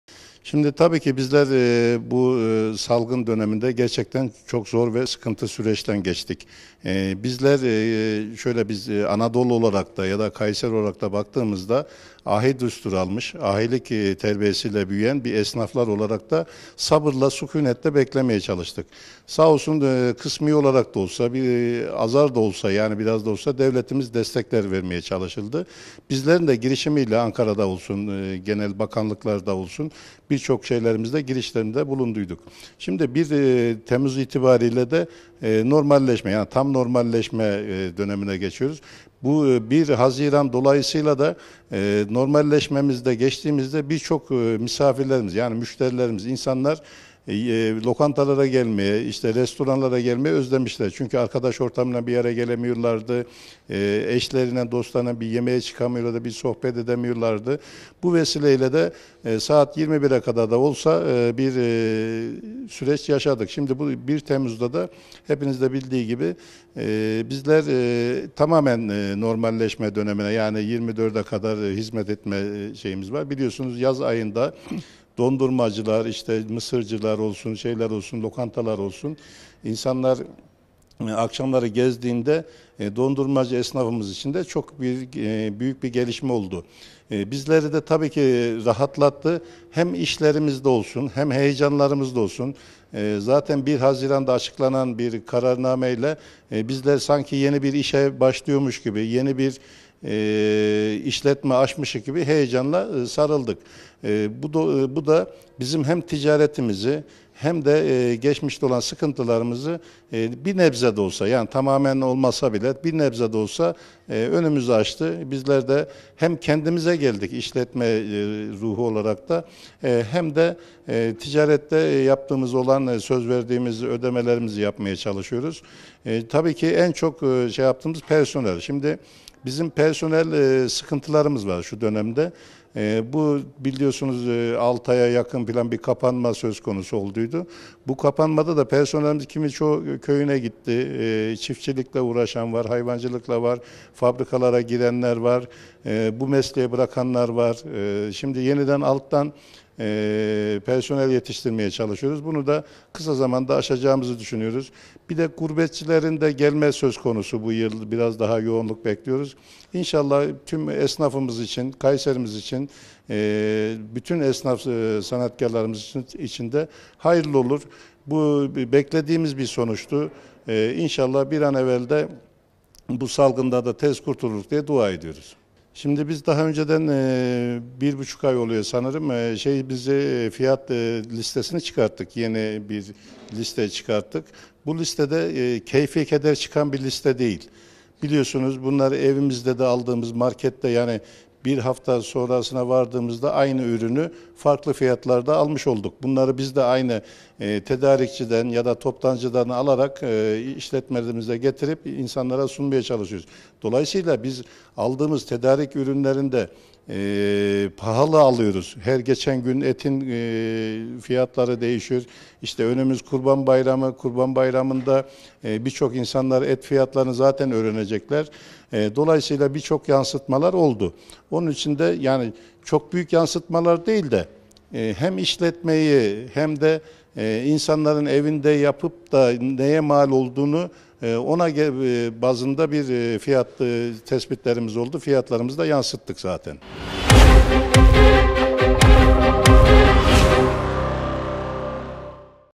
Lokantacılar 1 Temmuz’dan Umutlu
KAYSERİ,(BGC)-“Kontrollü Normalleşme” sürecinde 3’üncü Etap’ın duyurulmasıyla birlikte yeme-içme sektöründe iyileşme beklentileri hız kazandı. Kayseri Lokantacılar ve Pastacılar Odası Başkanı Altan Aydemir, yeme-içme sektöründeki son durumun alınan yeni kararlar ile birlikte daha da düzeleceğini belirtti.

İçişleri Bakanlığının 27 Haziran 2021 tarihli “Kontrollü Normalleşme 3. Etap” konulu genelgesine göre 1 Temmuz 2021 Perşembe tarihinden itibaren yeme-içme mekanlarında kağıt ve taş ile oyunların oynanmasına yönelik kısıtlamalar, masa ve sandalyelere yönelik sayı kısıtlaması mesafe kurulmasına uyulması kaydıyla kaldırıldı. Bahsi geçen mekanlarda kaldırıldı. Yeme-içme mekanları ile konaklama yerlerinde canlı müziğin saat 24.00’e kadar yapılabileceği belirtildi.

Yaşanan süreç üzerine değerlendirmede bulunan Aydemir, “Bu salgında çok sıkıntılı bir süreçten geçtik. Bizler Ahi düsturu almış esnaflar olarak sabır ve sükûnetle bu süreci geçirmeye çalıştık. Devletimiz destekler vermeye uğraştı. 1 Temmuz itibarıyla yeni bir normalleşme dönemine giriyoruz. Haziran başından beri müşterilerimizin yeme-içme mekanlarına gelmeyi özlediğini gördük. Çünkü insanlar sosyalleşemiyordu, arkadaş ortamlarında, eş-dost ortamlarında bir araya gelemiyorlardı” şeklinde konuştu.

Kısıtlamaların giderek gevşetilmesiyle birlikte sektörlerinin canlandığını, ancak çalışan sıkıntısı çektiklerini belirten Aydemir, “Haziran’da alınan kararlarla birlikte iş yerimizi yeni açmışçasına işlerimize sarıldık. Bu da geçmişte olan sıkıntılarımızın bir nebze önünü açtı. Bizler de hem kendimize geldik, hem de söz verdiğimiz ödemelerimizi yapmaya çalışıyoruz. Şu an personel sıkıntımız var. Kapanma dönemlerinde personelimiz köyüne gitti, çiftçilikle uğraşanlar oldu, fabrikada işe girenler oldu, bu mesleği bırakanlar oldu. Şimdi temelden personel yetiştirmeye çalışıyoruz. Bunu da kısa zamanda aşacağımızı düşünüyoruz. Gurbetçilerimizin de gelmesiyle işlerimizin daha da açılacağına inanıyoruz. İnşallah kararlar bütün esnaf ve sanatkarlarımız için hayırlı olur” ifadelerini kullandı.

“YENİ FİYATLAR KEYFE KEDER BELİRLENMEDİ”

Aydemir, Kayseri Lokantacılar ve Pastacılar Odası tarafından iki ay önce duyurulan yeni fiyat tarifesi hakkında “Bu listedeki fiyatlar keyfe keder belirlenmedi. Tedarik ettiğimiz ürünleri pahalıya alıyoruz. Her geçen gün et fiyatları değişiyor. Birçok insan et fiyatlarını Kurban Bayramı’nda öğrenecek. Bunlar çok büyük yansıtmalar değildir. Bundan sonra ise fiyat artışı söz konusu değildir” diyerek,  konuşmasını tamamladı.